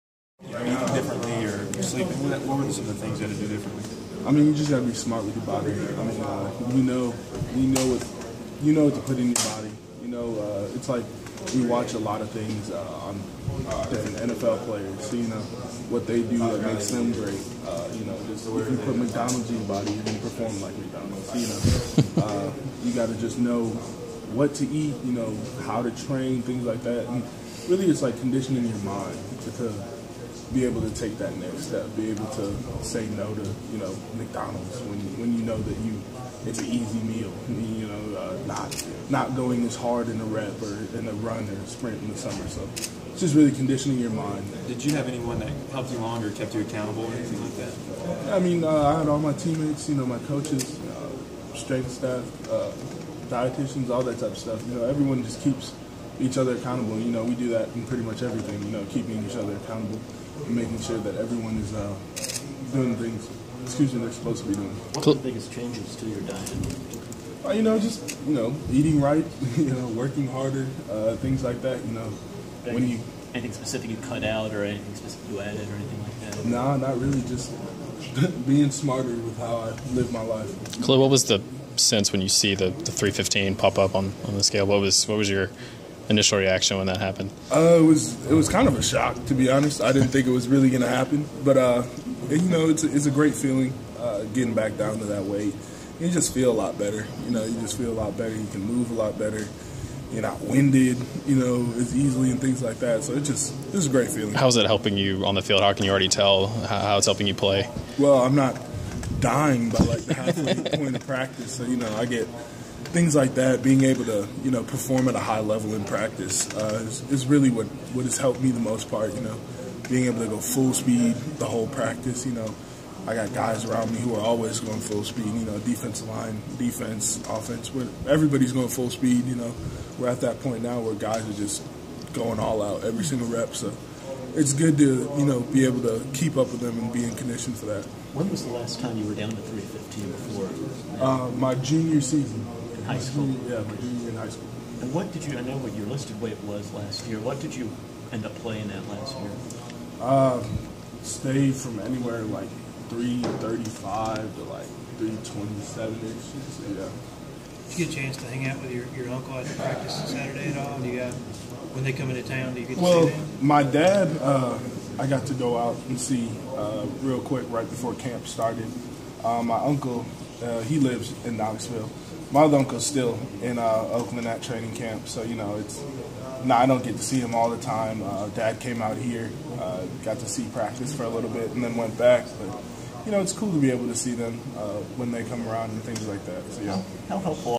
Kahlil McKenzie -- 8.9.16
Tennessee defensive tackle Kahlil McKenzie on how he changed his mindset to lose enough weight to reach his goal of playing his sophomore season at 315 pounds.